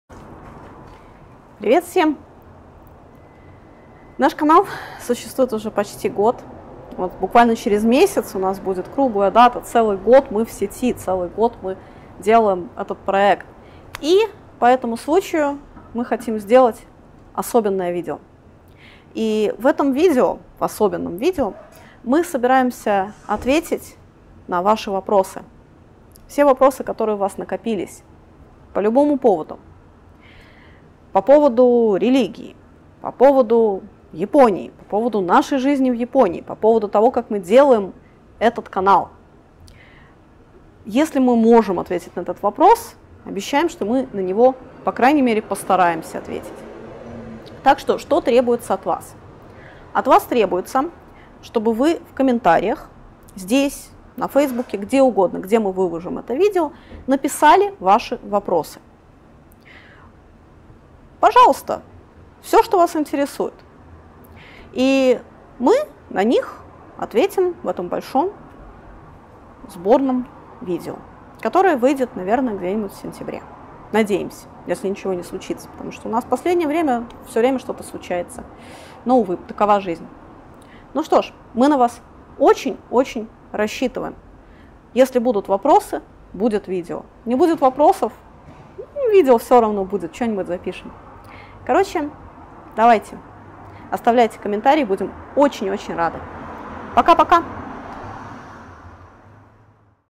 Нам нужны ваши вопросы!
Каналу скоро исполнится год! По этому случаю мы предлагаем вам, дорогие зрители, задать нам любой вопрос - и мы непременно постараемся на него ответить!